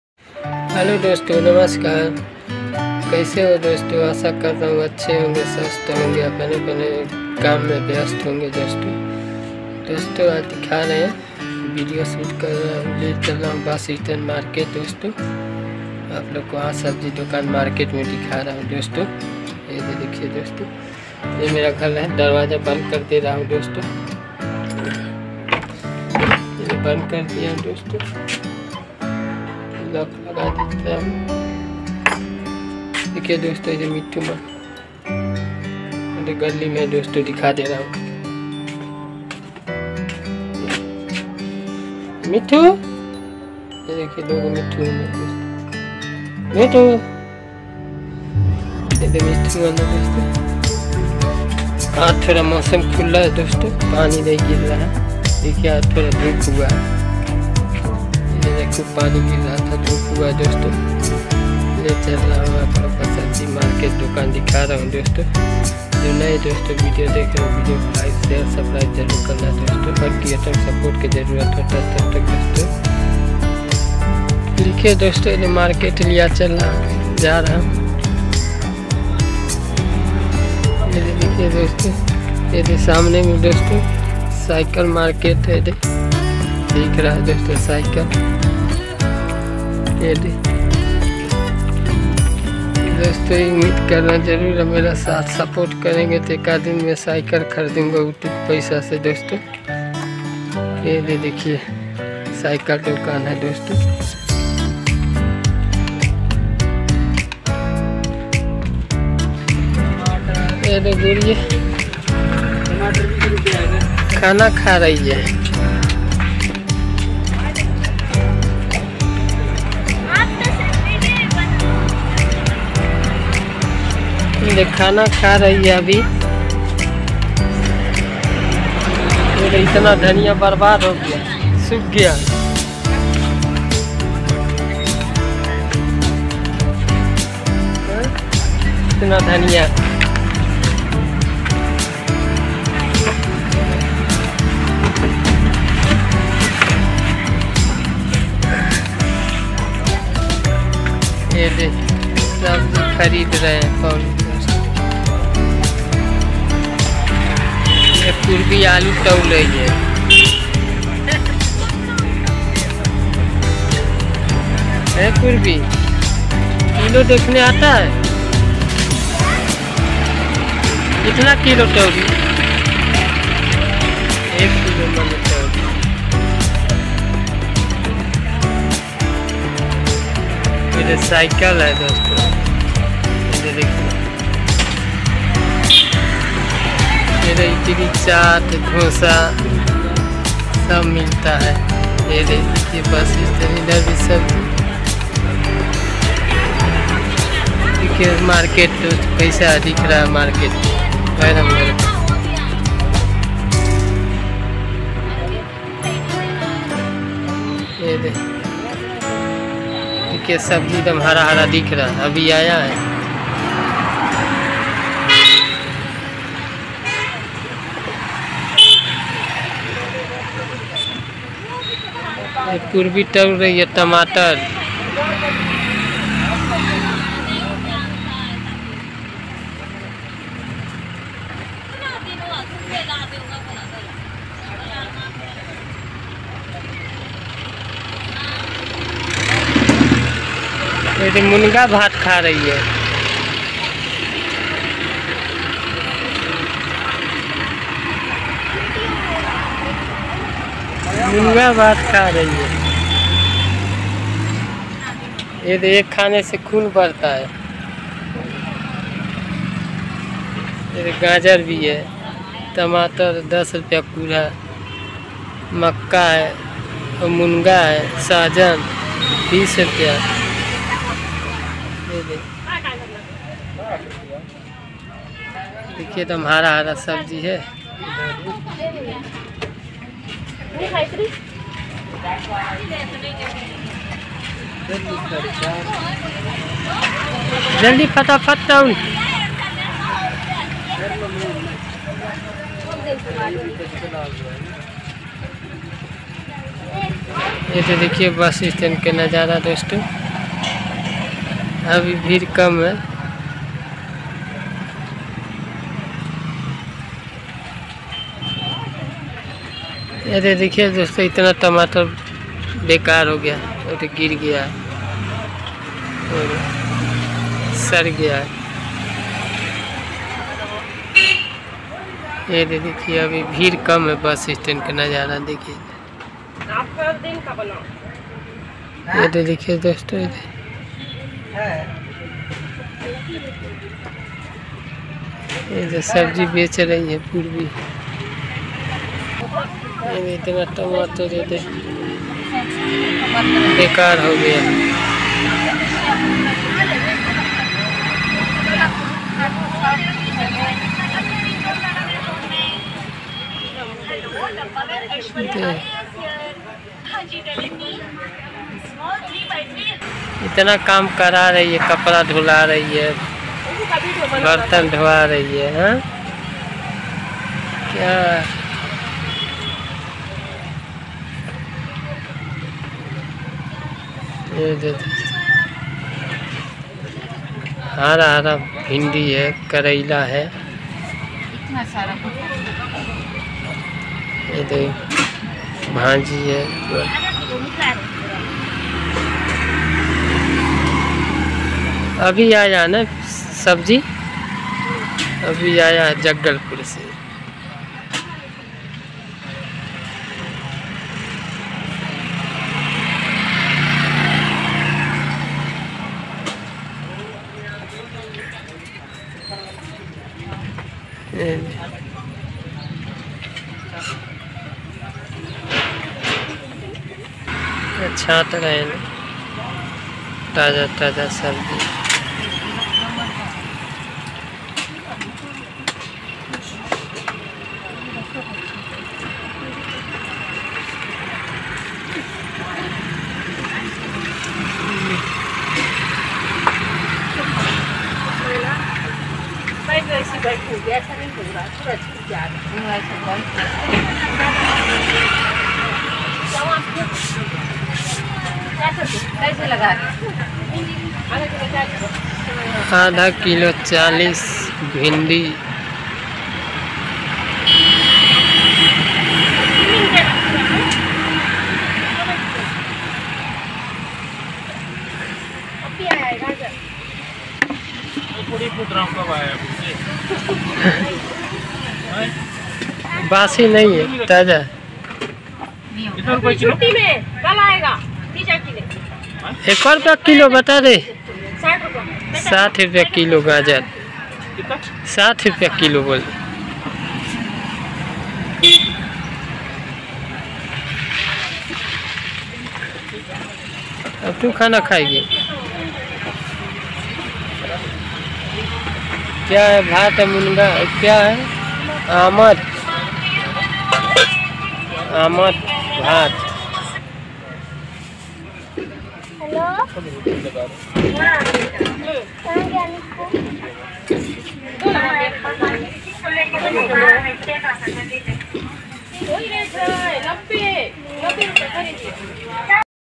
बस इतने में सब्जी बेच के बच्चे खाना खा😭🙏shortsyoutube
bus stand mein sabji bech ke khana kha rahi hai bacchi 🙏🙏🙏😭😭😭😭🙏😂😂🙏🙏shortsyoutube